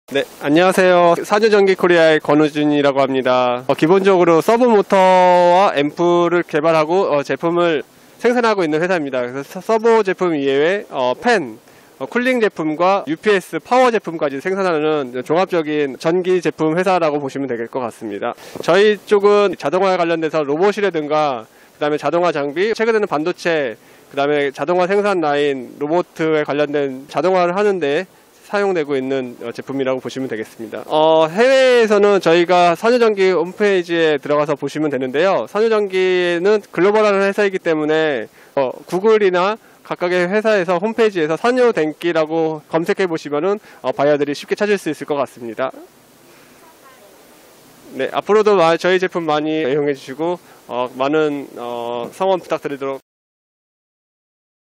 [세미콘 코리아 2022] 자동화 관련 전기 제품 - 산요전기
산요전기사의 자동화 관련 전기 제품이 세미콘 코리아에 소개되었습니다.
1987년 189개 부스로 시작한 세미콘 코리아는 한국 반도체 산업의 지속적인 성장에 힘입어, 반도체 산업을 대표하는 전시회로 공고히 자리매김했습니다.
세미콘 코리아 2023는 국내외 반도체 재료 및 장비 업체들이 참여하는 전시회로, 2023년 2월 1일부터 3일까지 서울 코엑스에서 개최됩니다.
최신 반도체 재료, 장비 및 관련 기술을 선보이는 전시회를 주축으로 반도체 기술 심포지엄, 마켓 트렌드 포럼, 구매상담회, 표준 등을 통해 업계 최신 정보를 교환할 수 있는 장을 마련했습니다.
이를 통해 글로벌 반도체 장비 업계의 현재와 미래를 점검할 수 있는 실질적이고 전략적인 기회가 될 것입니다.